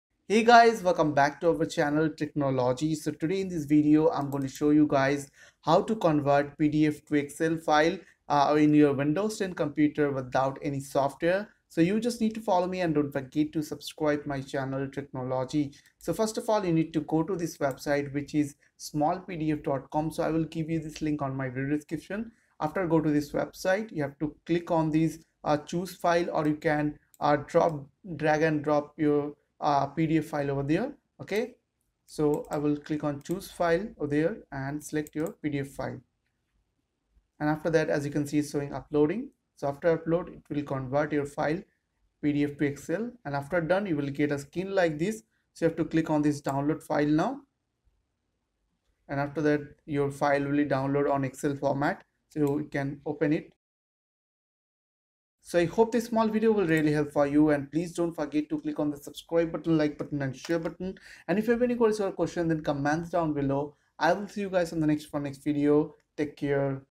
How To Convert PDF to Excel Without Any Software
In this video, I will show you guys how to convert PDF to Excel files without any software for free.

1. Mic
2. Microphone Stand
3. Headphone
4. Phone
5. Strip LED Light
6. Camera